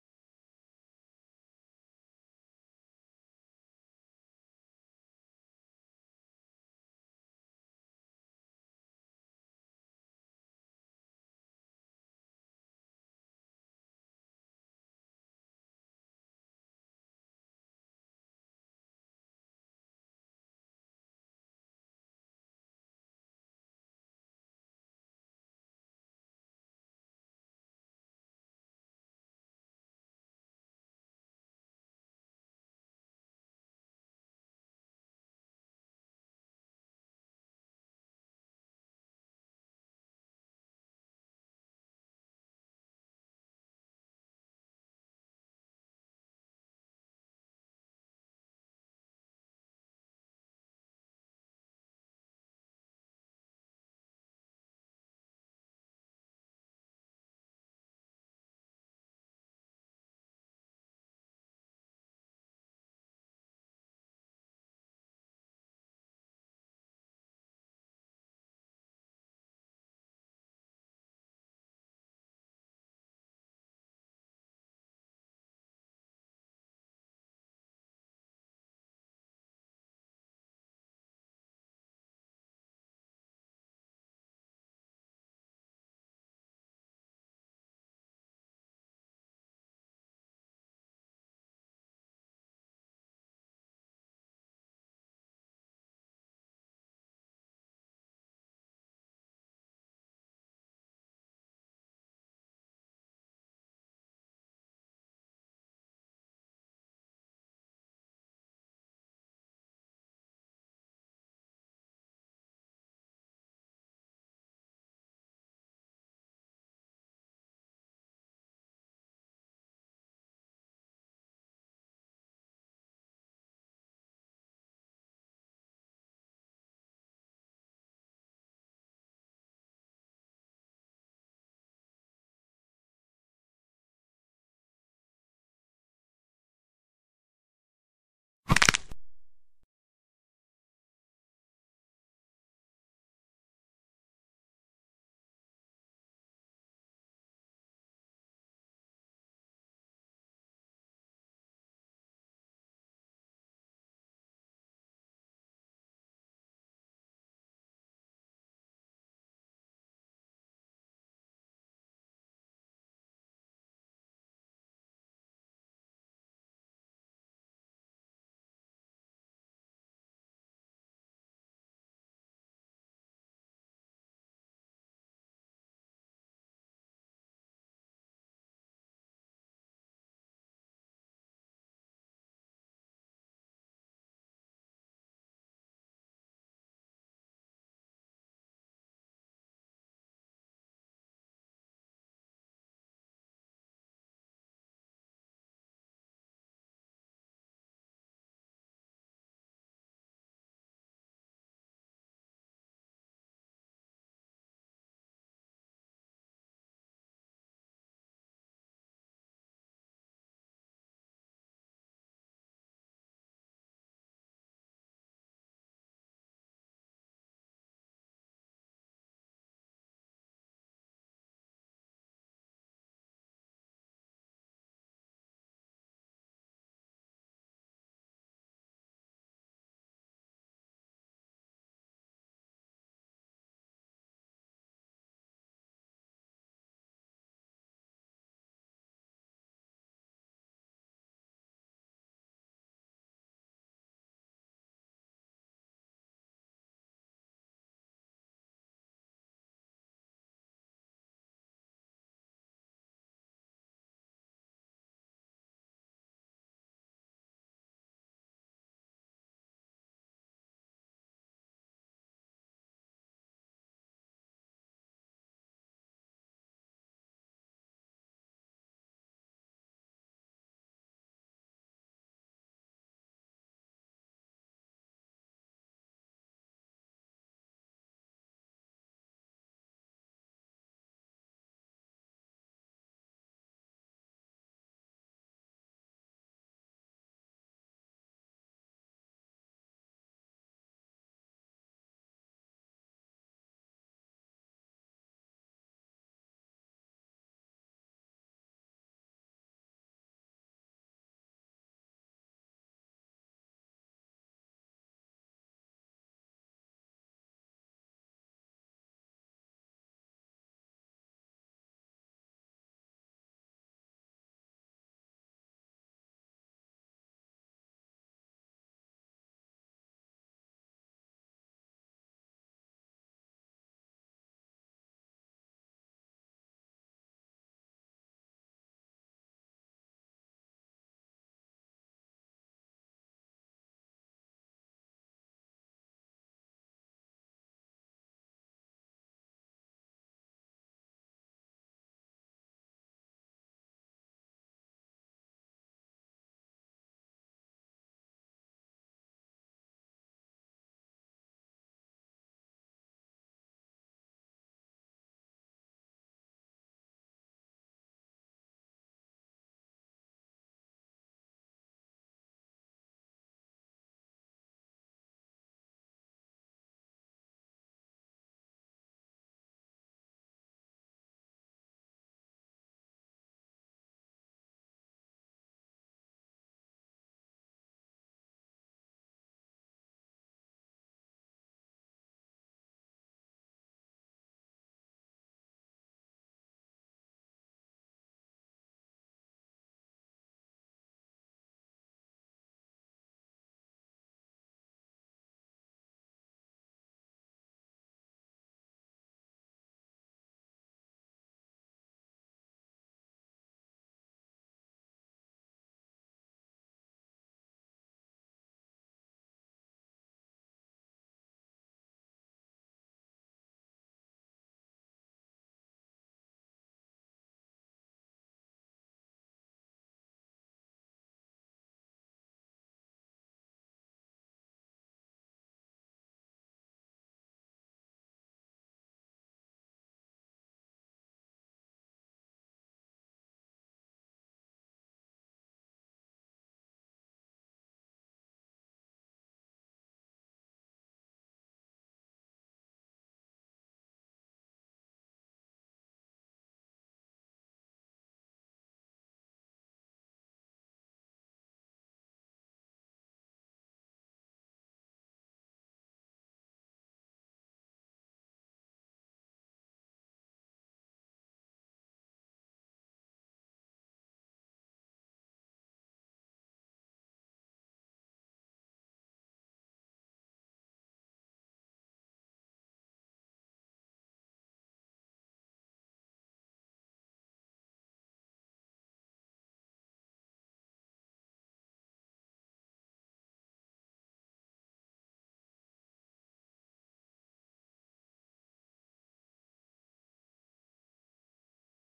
Past Aftons meet their future self//part 1// Gacha Club//+Bonus scene//
This is my AU for now-